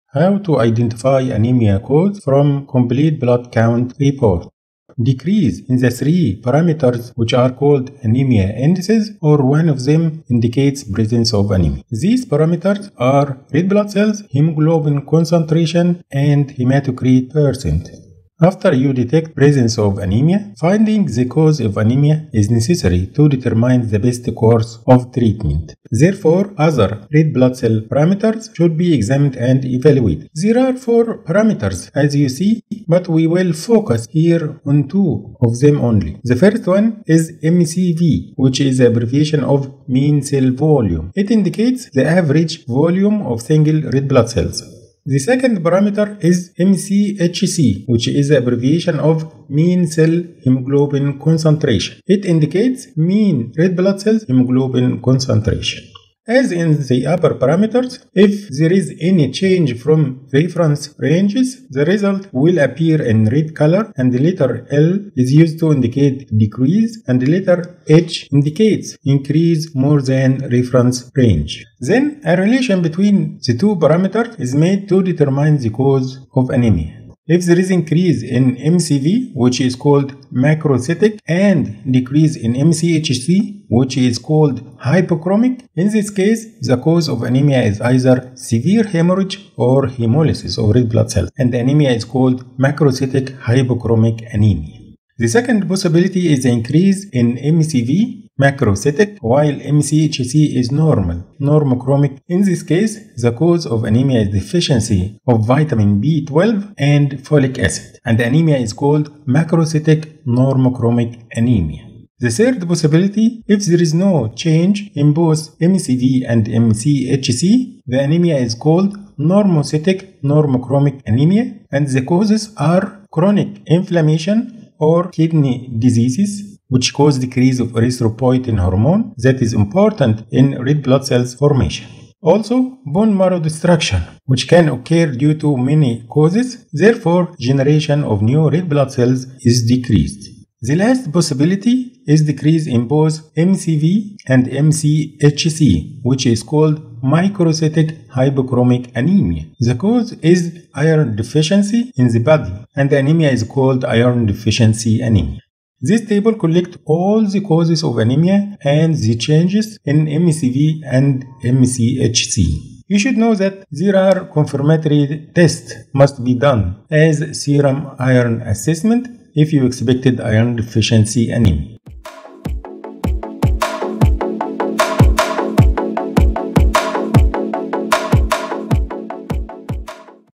How to identify anemia's cause from a CBC report.
by evaluating MCV and MCHC you can detect the cause of anemia
such as iron deficiency anemia
hemorrhagic anemia
hemolytic anemia
anemia of the chronic diseases
megaloblastic anemia
aplastic anemia